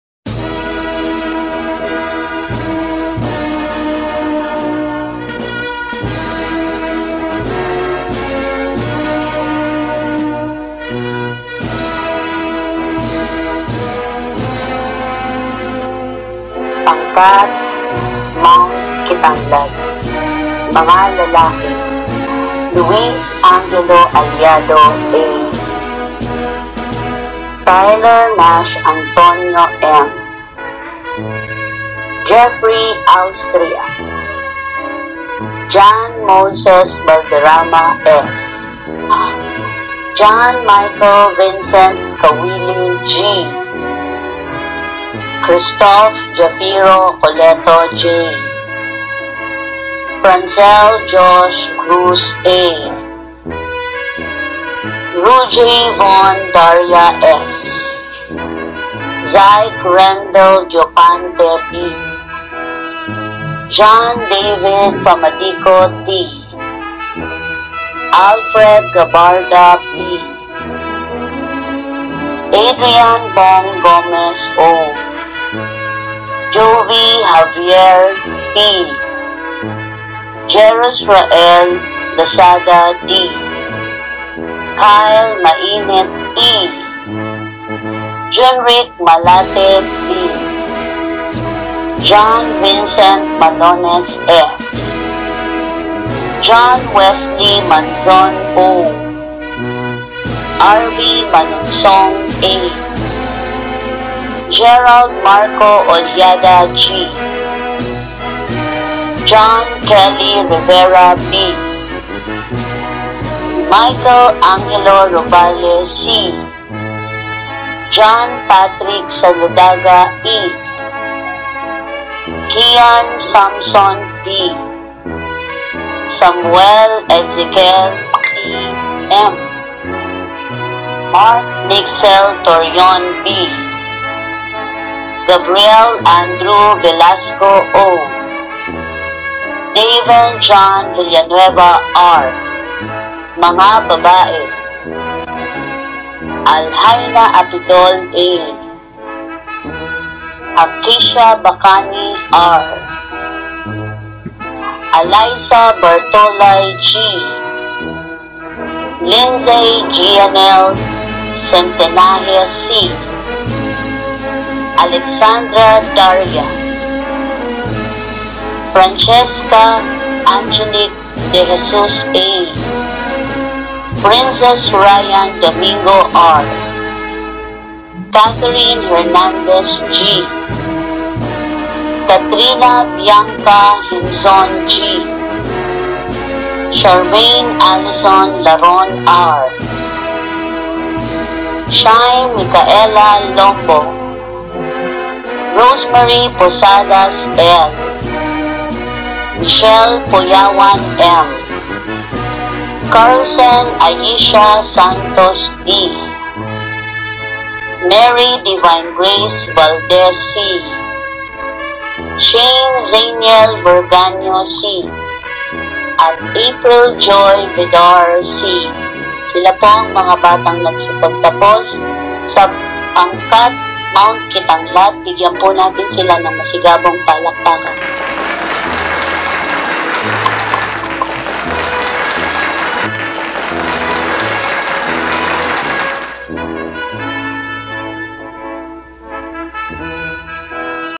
e-Graduation (Mt. Kitanglad)
It's Ma'am Cecil's graduation tribute for her pupils in grade six Mt. Kitanglad this time of ECQ.